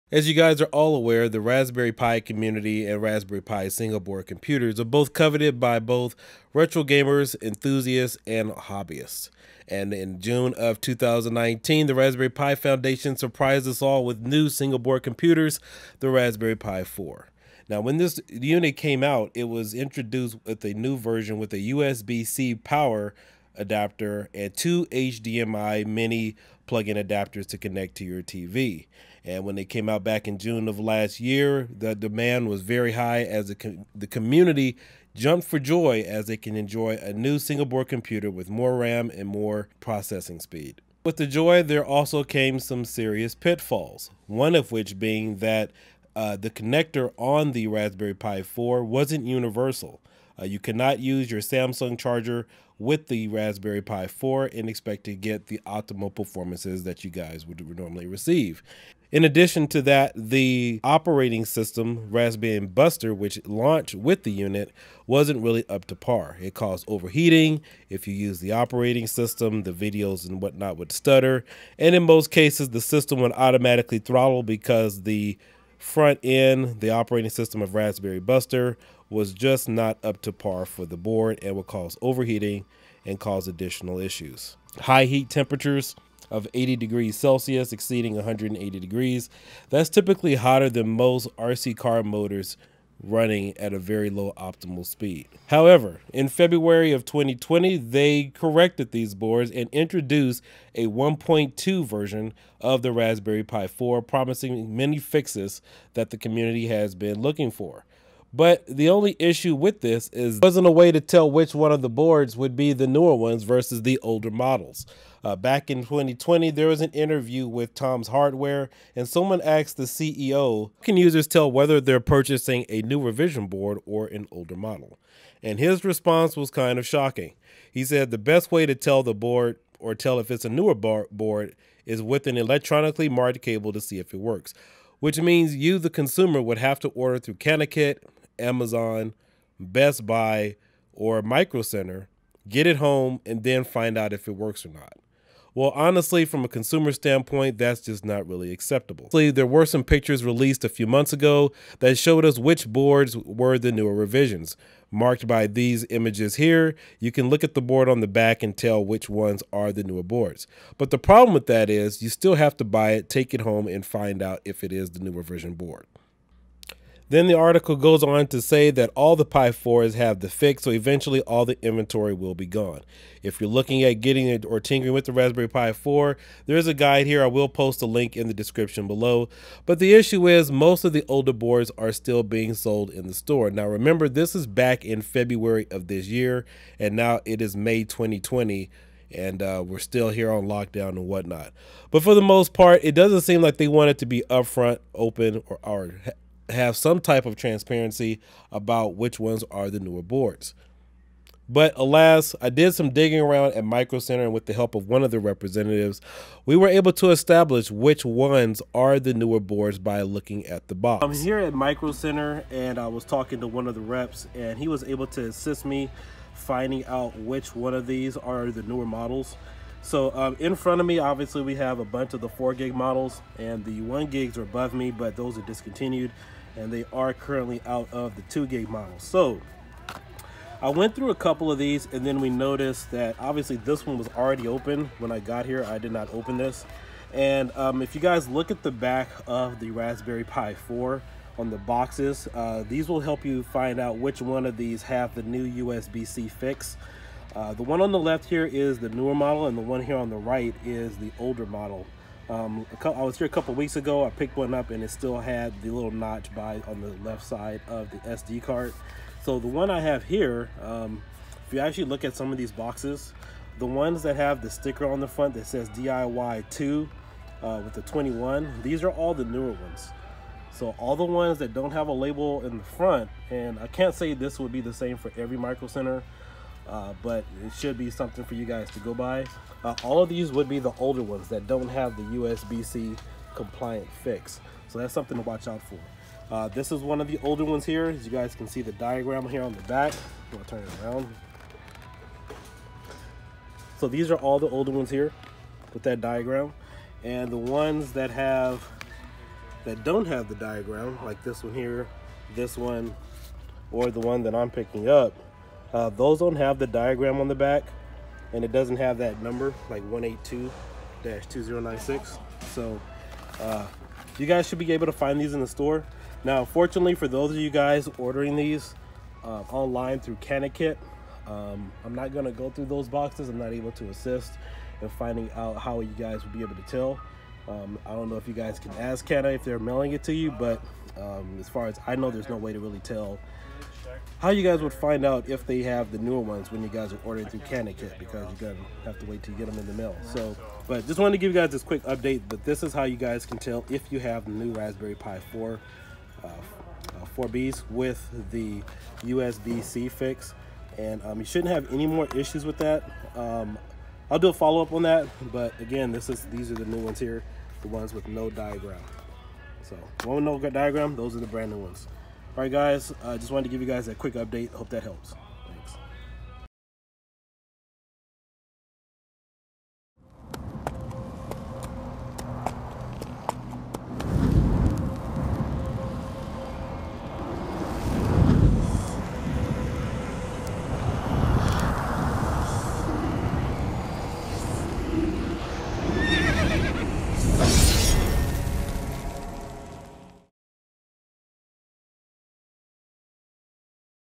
Raspberry Pi4B 1.2 Version Usb C New Compliant Fix
Raspberry Pi 4 B revision 1.2 how to tell the difference with the Usb C Compliant fix.

++Use coupon KIODIEKIN10 at checkout to get $10 off your order++
++Xsplit Desktop Recording and 4k Streaming Software++

Equipment I Use:

"The Only Memory" Cards I use Samsung Evo

Samsung Solid State Drive